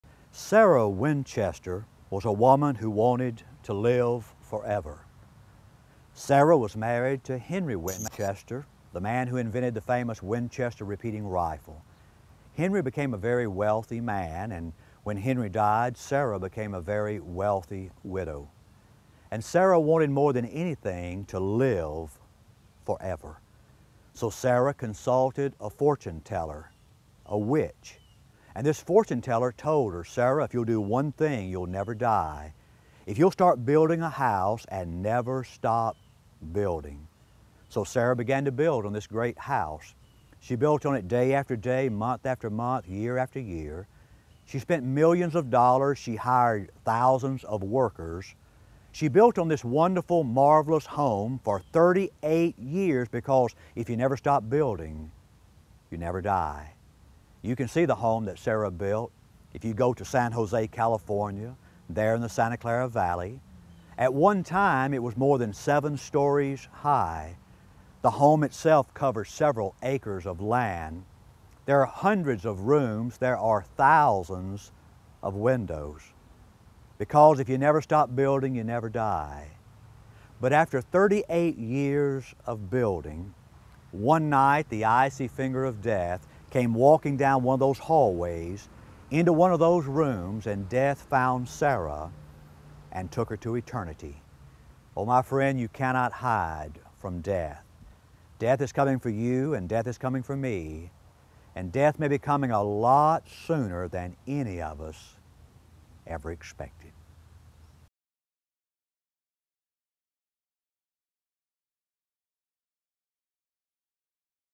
YOU CAN NOT LIVE FOREVER! The true story of Sarah Winchester.
YOU CAN NOT LIVE FOREVER! The true story of Sarah Winchester.  Sarah Winchester was a woman who wanted to live forever.  This 2 minute video tells her amazing story.